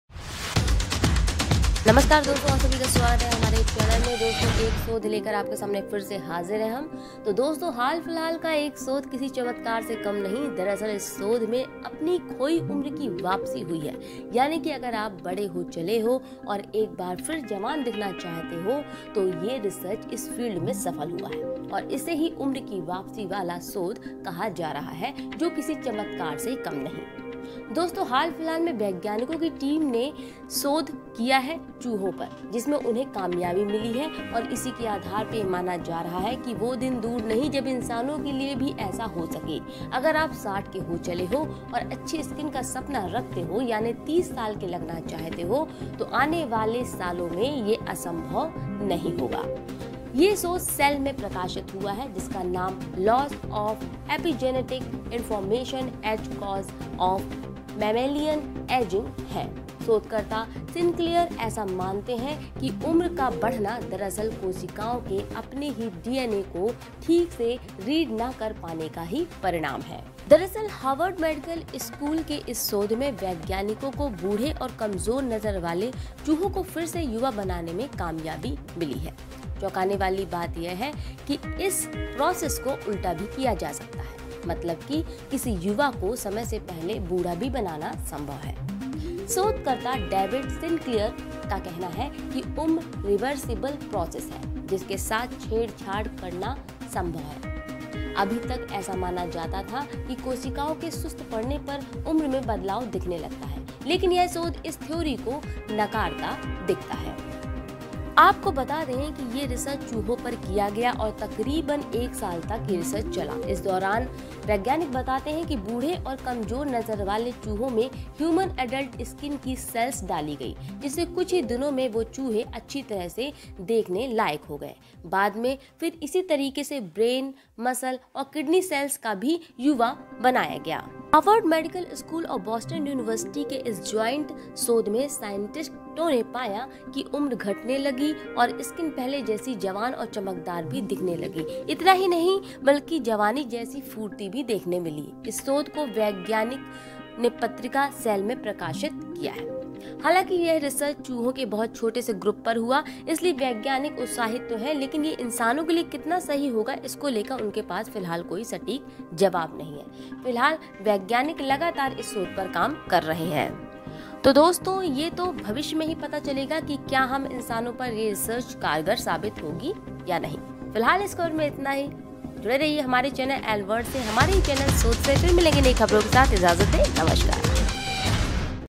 Scientist Discovers Aging Clock to Speed and Reverse Aging | Human age can be restored
Scientist Discovers Aging Clock to Speed and Reverse Aging | Human age can be restored वैज्ञानिक सफल, उम्र की वापसी हुई, चमत्कारी रिसर्च।